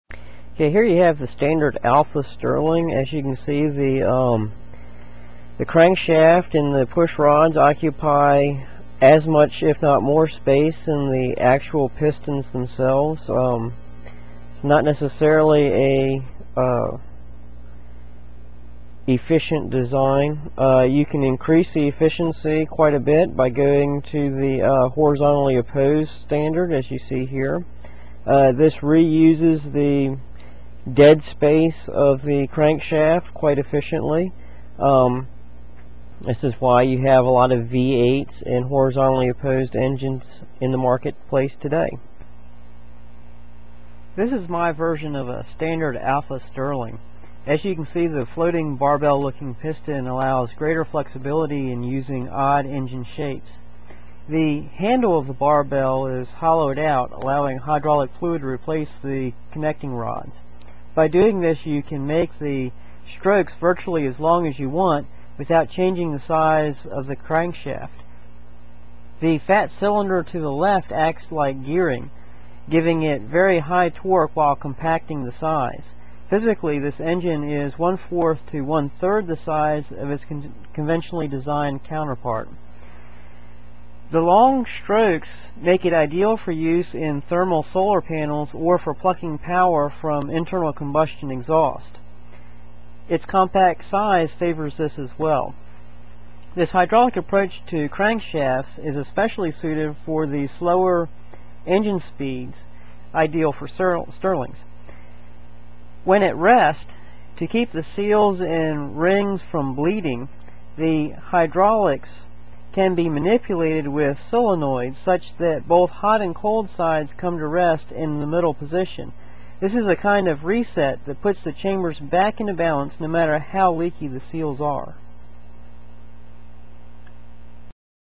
The HHOPP engine
A way of building high displacement engines in incredibly small packages. (FREE to READ) Also works for ICs, diesels, compressors, conditioners, etc.